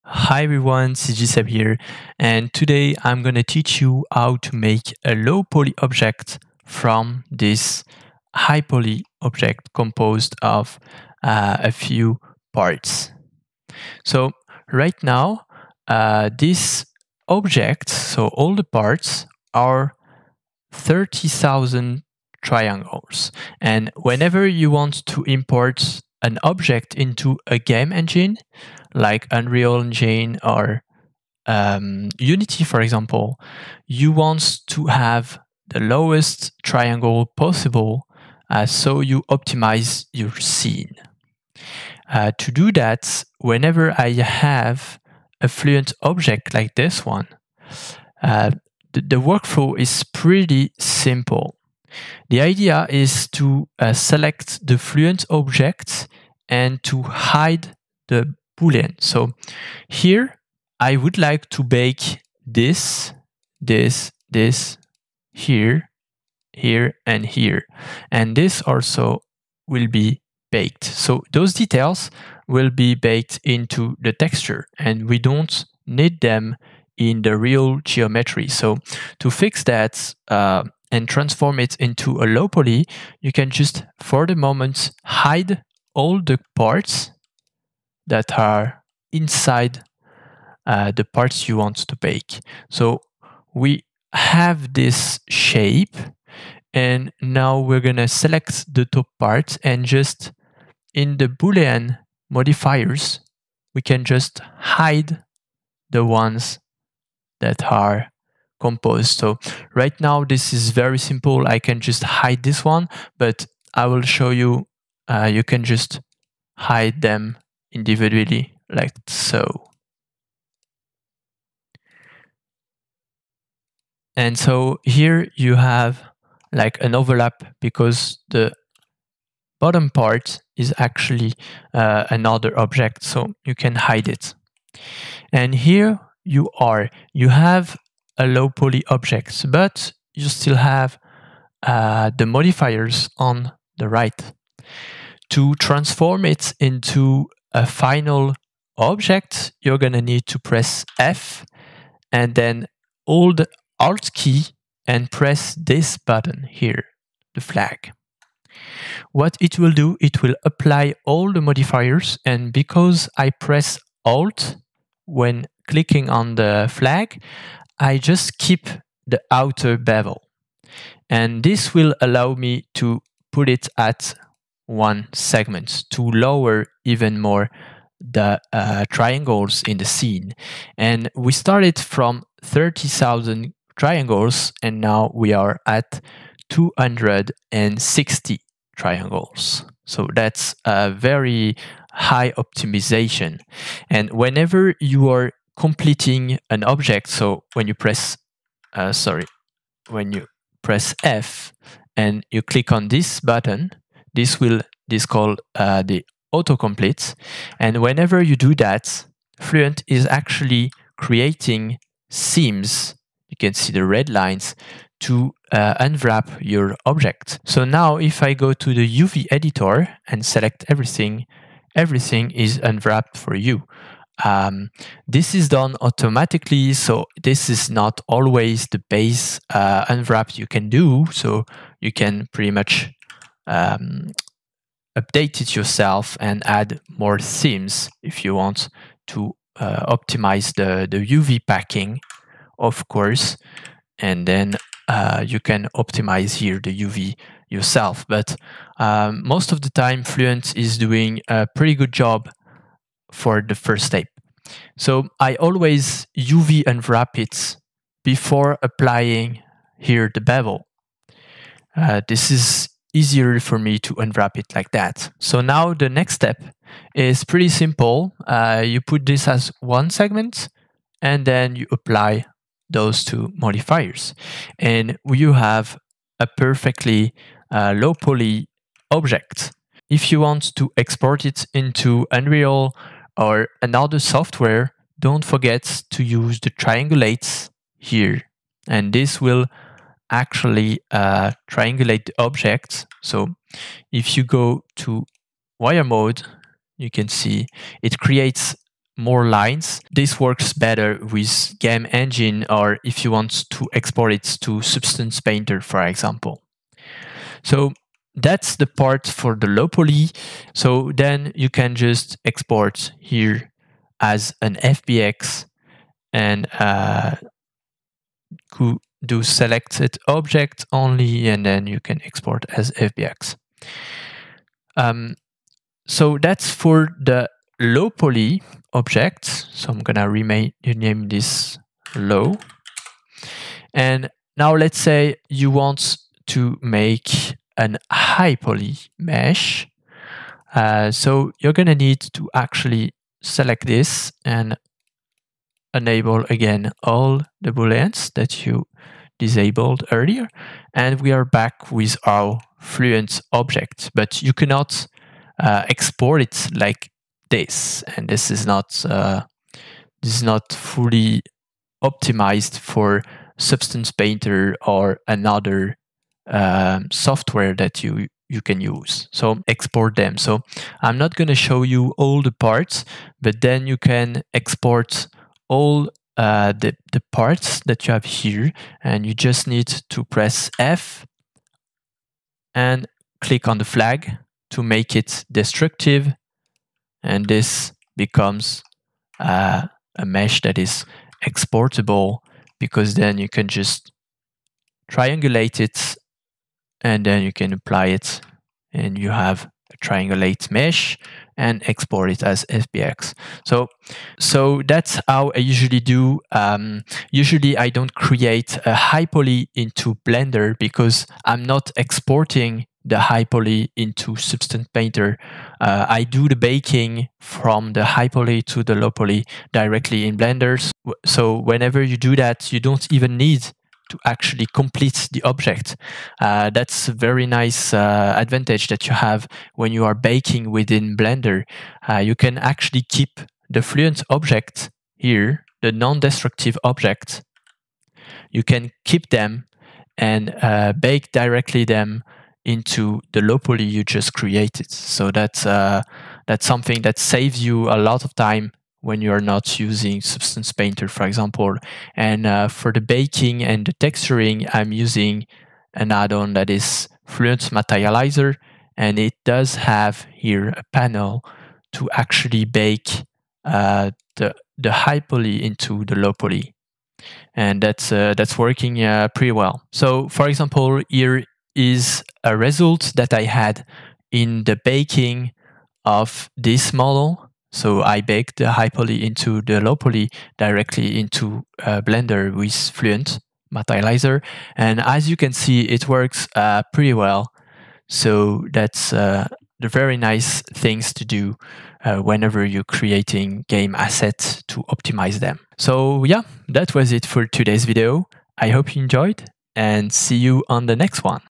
From High To Low Poly Never Been This Easy - Fluent (Blender Addon)
Hi everyone! Here is how to make a low poly from a high poly done in Blender with Fluent 2.0.

Follow me for more Blender and Sci-Fi related tutorials :)

Graphics Card: RTX 3070
CPU: i5 10600K
RAM: 16GB
Blender: 3.1
DECALMachine: 2.6
Fluent: 2.0.4
Fluent Materializer: 1.1